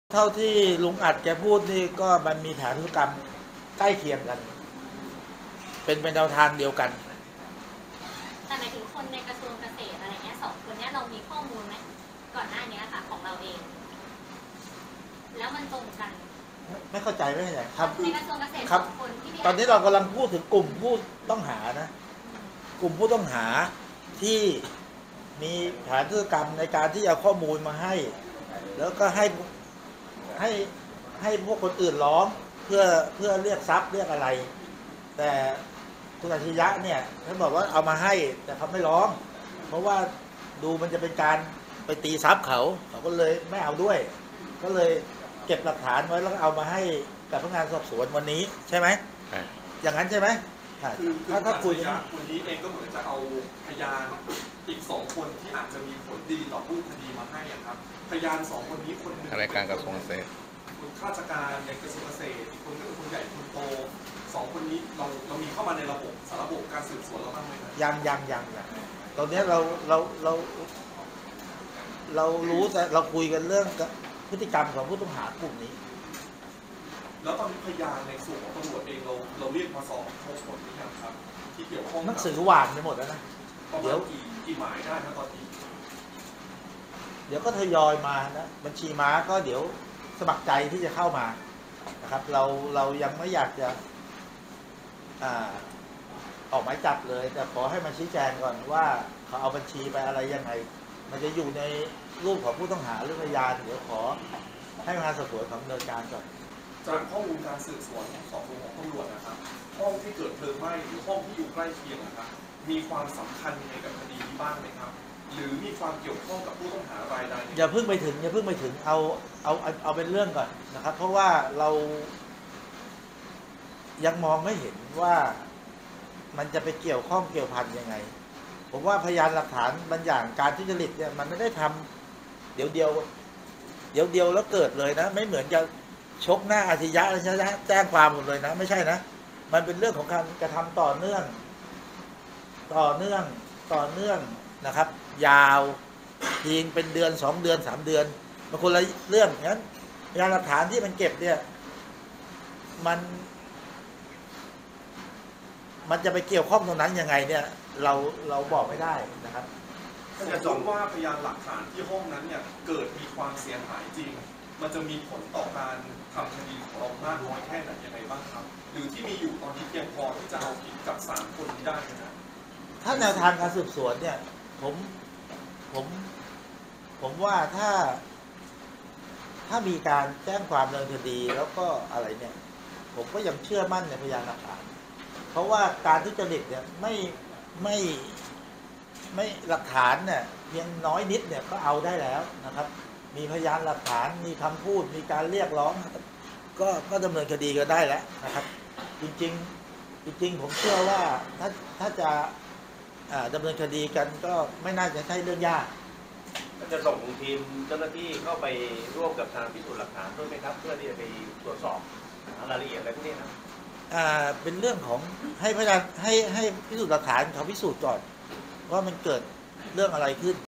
"บิ๊กเต่า" ไม่กังวลเหตุไฟไหม้ ก.เกษตรฯ ผบช.ก.สั่งกองปราบช่วยทำคดี
"บิ๊กเต่า" เผยเหตุไฟไหม้กระทรวงเกษตรฯ ไม่ส่งผลรูปคดีแก๊งศรีสุวรรณตบทรัพย์ เตรียมออกหมายจับเพิ่มอีก 1 ราย ผบช.ก.ตั้ง "รองหมู"เป็นหัวหน้าคณะทำงานให้กองปราบช่วยทำคดีหวั่นส่งฟ้องไม่ทัน